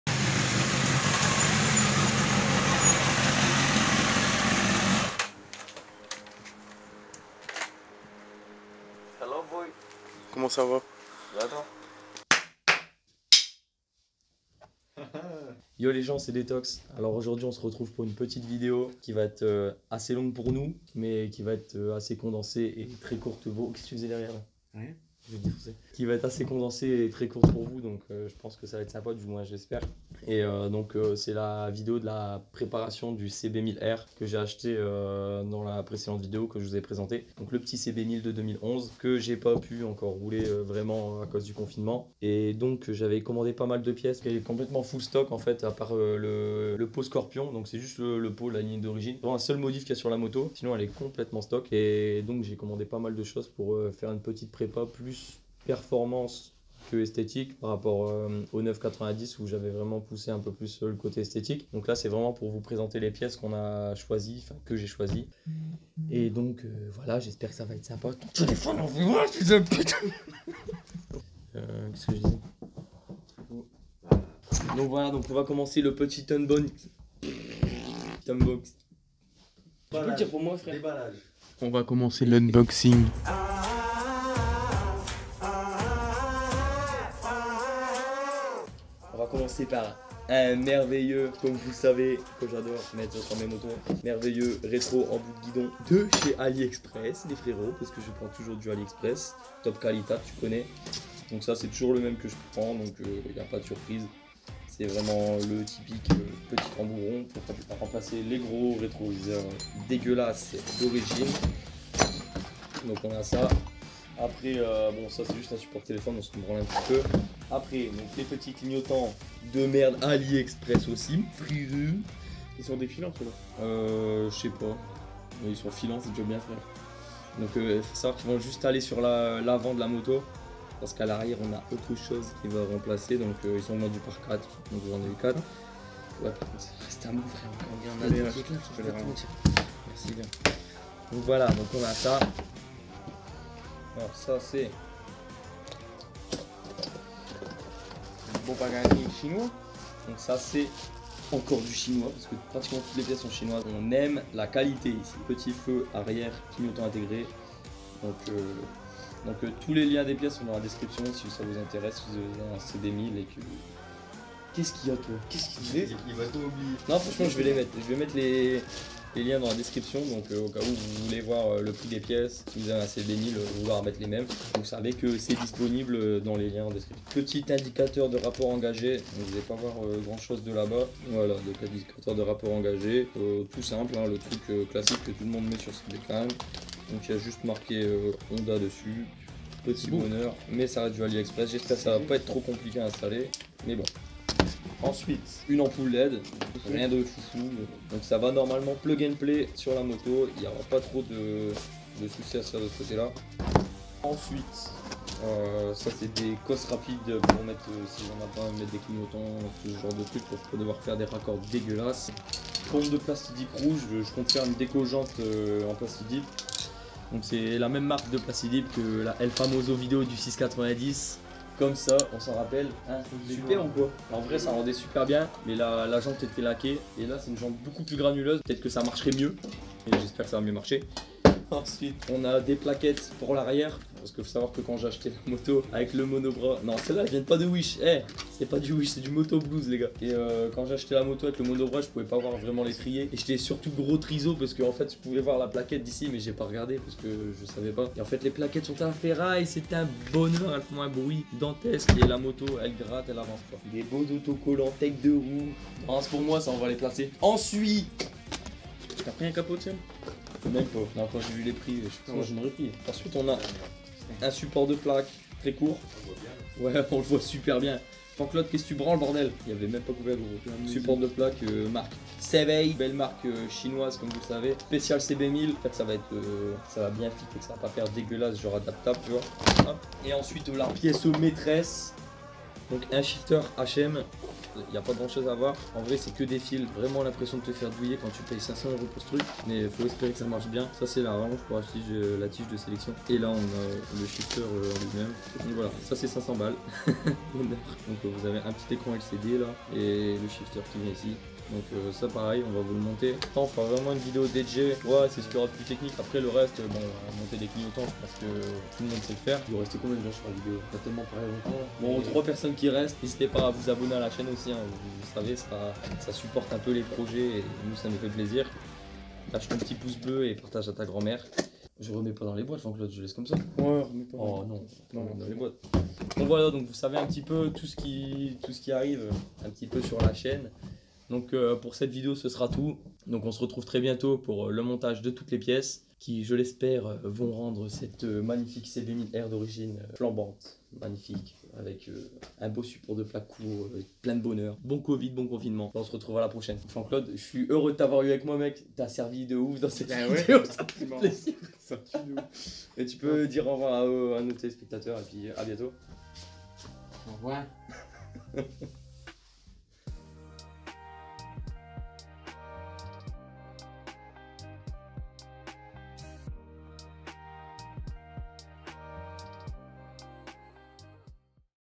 ON COMMENCE LA PREPA DU CB1000R ! | EP.1 LES PIECES !
Hey les boys! c'est parti pour la transformation du cb1000r de 2011 ! courte vidéo pour vous expliquer ce qu'on a prévu de faire sur cette moto et les prochaines vidéos qui viendrons sur la chaine en rapport avec cette moto ! restez branché !

pièces présentées dans la vidéo :
indicateur de rapport engagé

ampoule led :

support telephone :

feu arriere :

cosses rapides :

support de plaque :